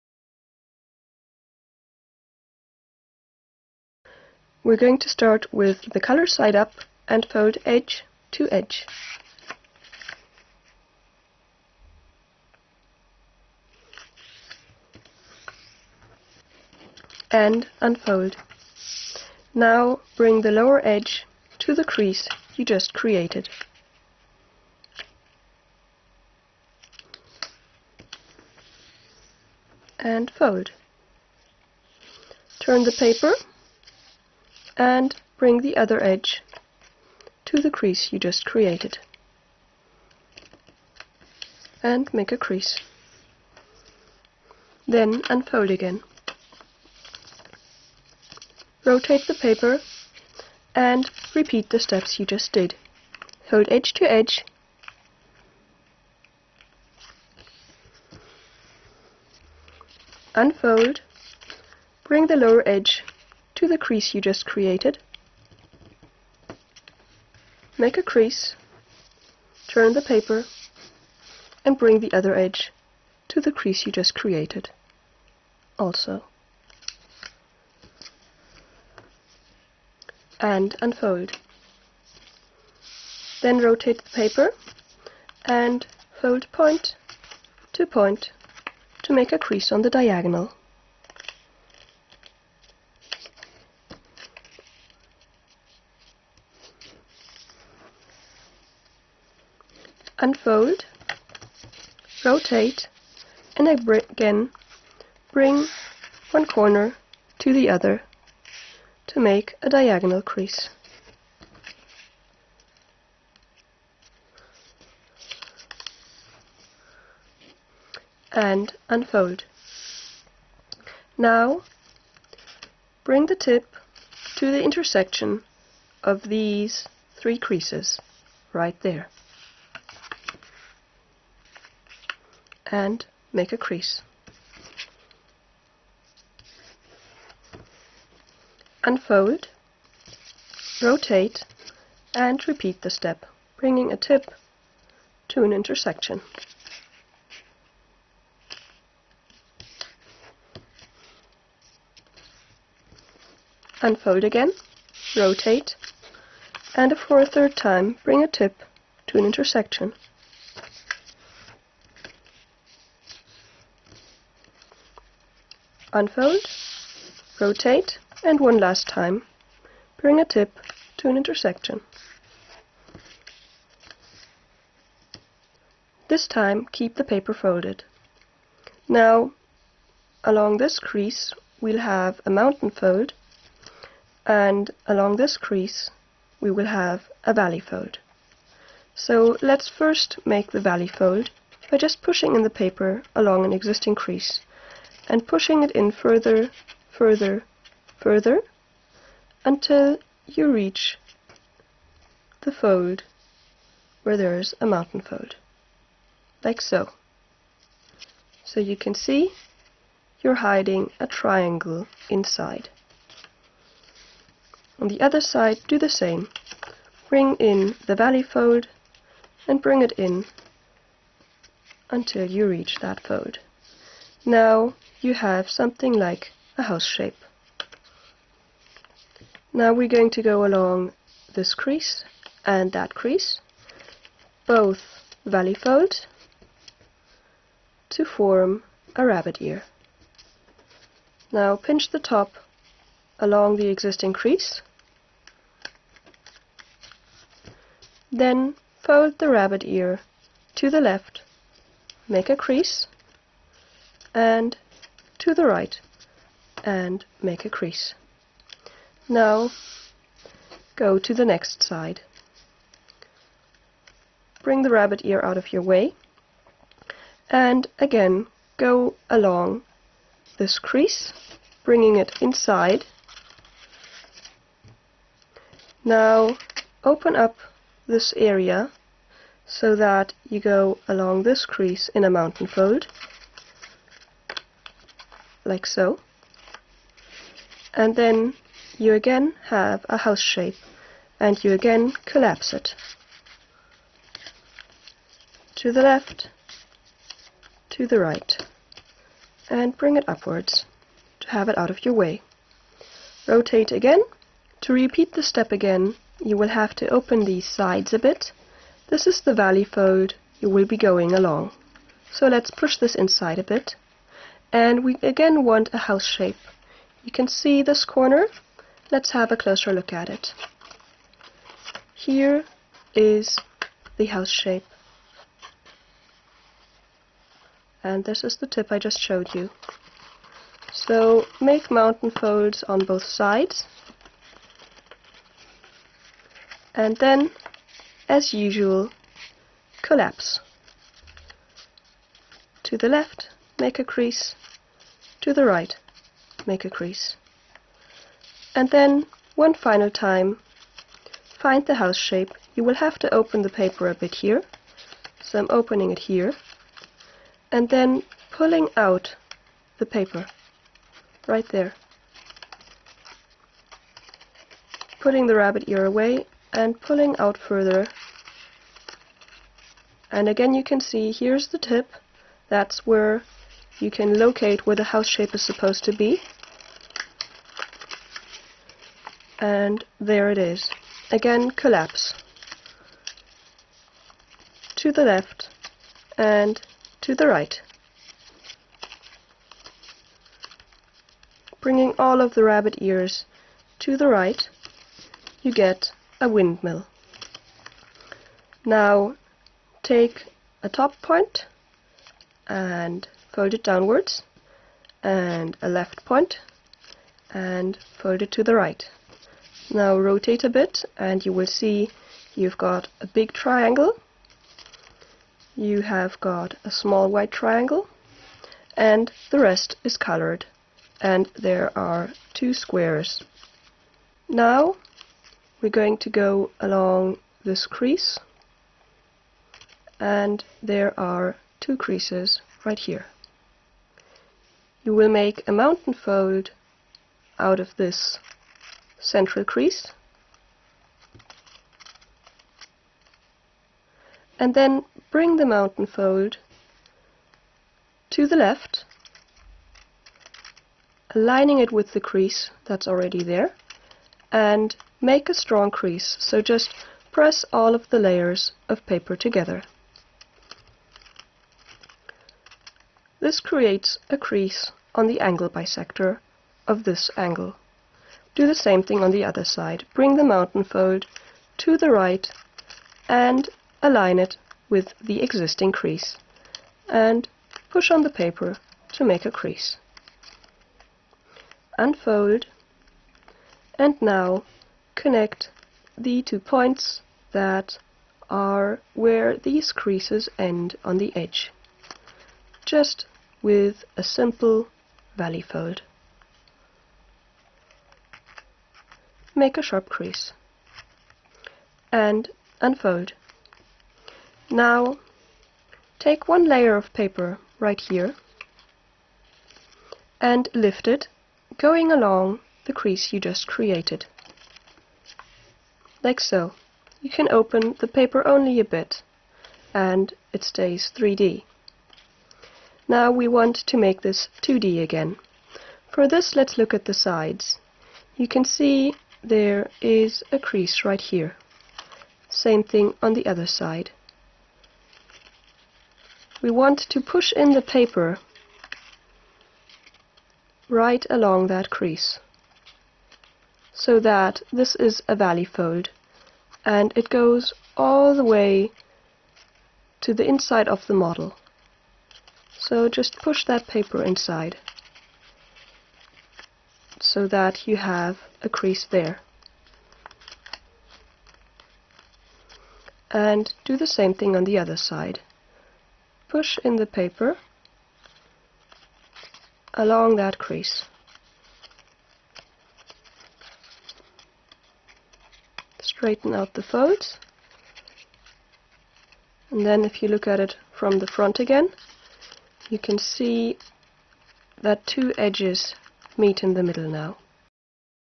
Origami Maple Leaf by "Jassu" Kyu-seok Oh (Part 1 of 5)
Paper size used in the video: 15cm square (6 inch)
Recommended paper size: 20cm square (8 inch)
I've finally found the time to do a remake of the instructional video on the maple leaf designed by "Jassu" Kyu-seok Oh. Jassu kindly gave me permission to make this video accessible to you.